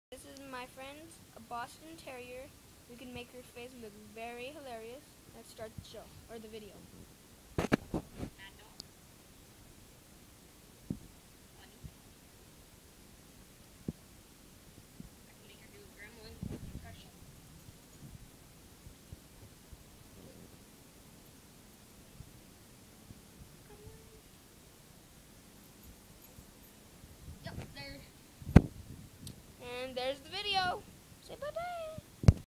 funny dog impressions
my boston terrier can do mad dog, bunny, and others.